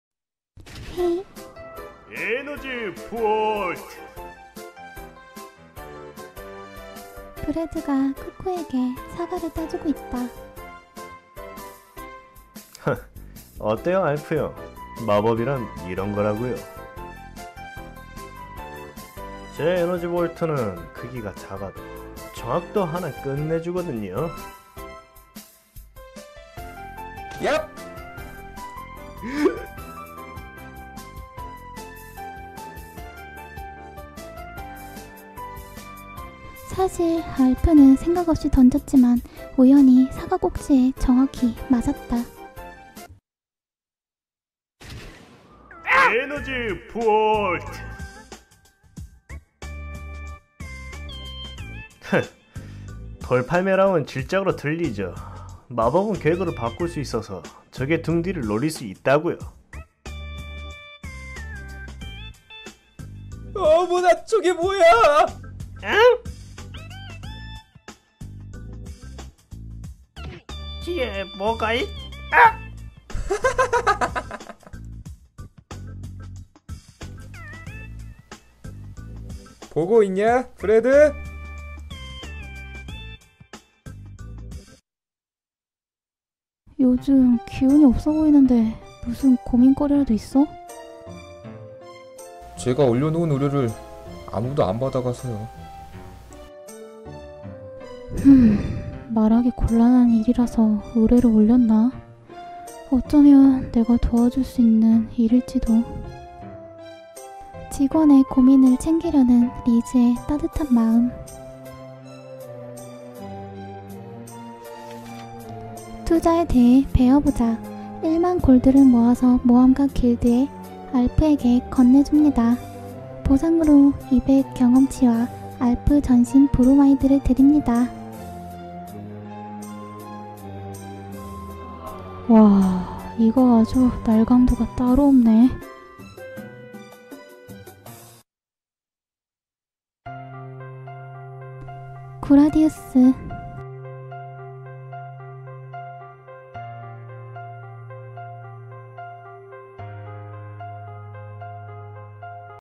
[달콤] 구라디우스 - 13화
달콤의 웹툰 더빙 구라디우스 - 13화 실황 영상!
구라디우스는 현재 투블루에서 연재되고 있는 웹툰입니다
작가님에게 허락받고 웹툰 더빙하고 있습니다
더빙 도움 - 좀비드래곤님,PD라파엘님

원작자(original author) : 해요(파스토조)